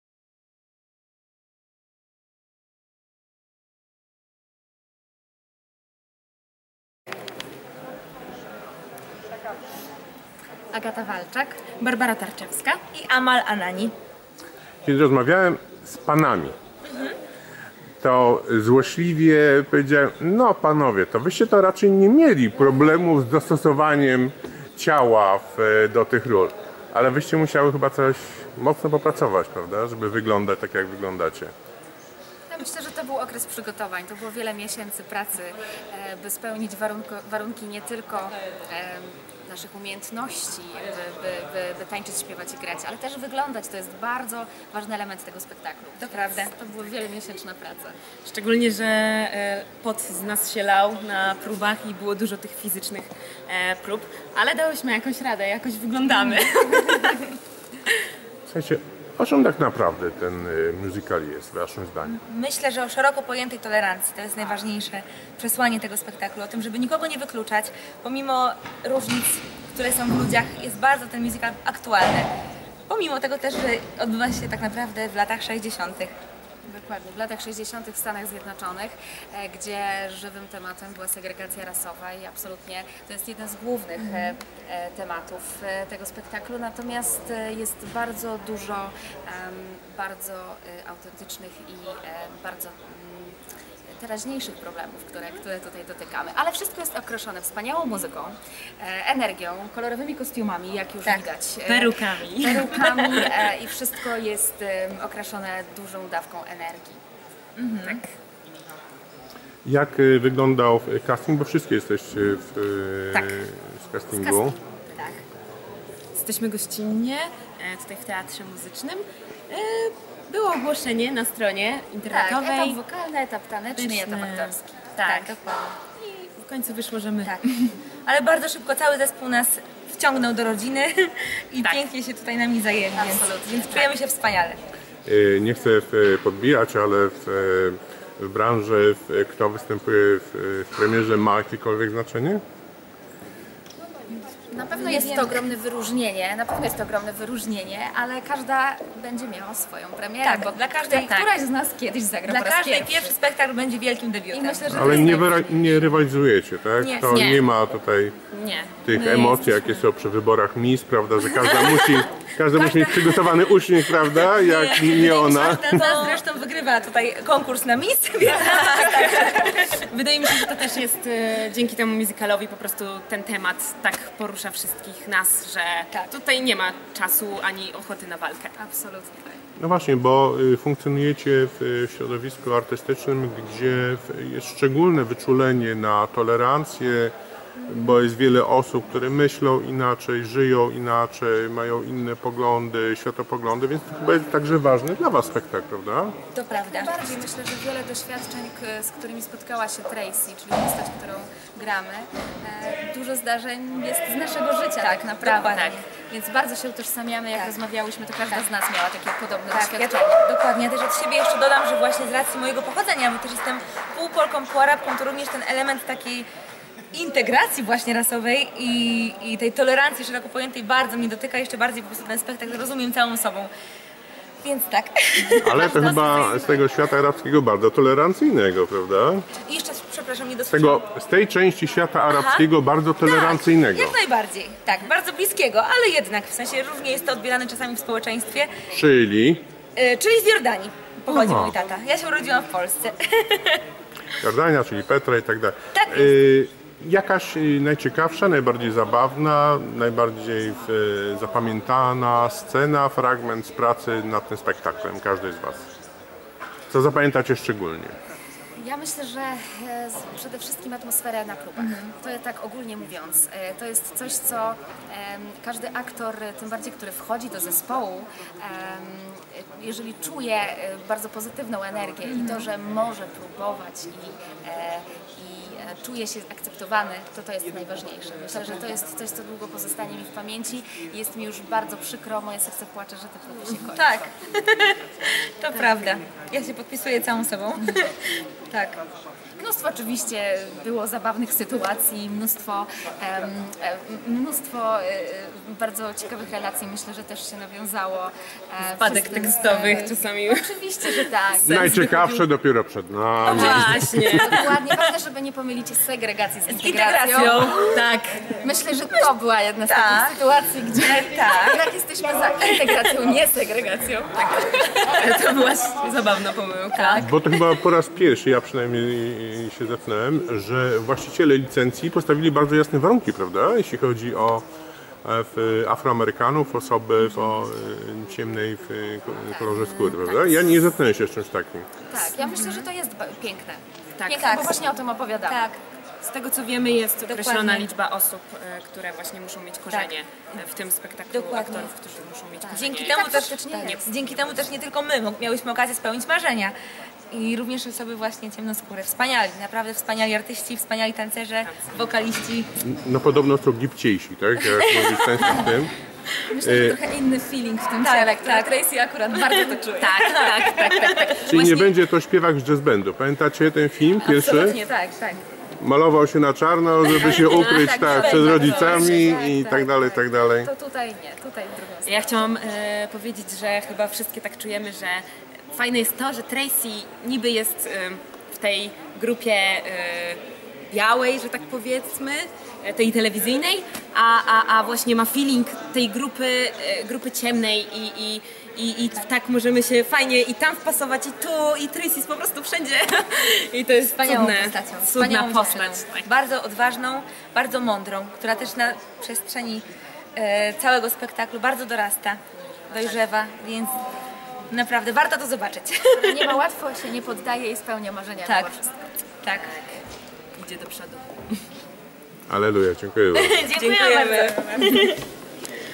Hairspray w Teatrze Muzycznym w Gdyni: mówią: A. Walczak, B. Tarczewska i A. Anani
W rolach Tracy Turnblad wystapią trzy debiutantki w glownych rolach na Dużej Scenie Teatru Muzycznego w Gdyni: Agata Walczak, Barbara Tarczewska i Amal Anani.

Próba medialna, 4/9/19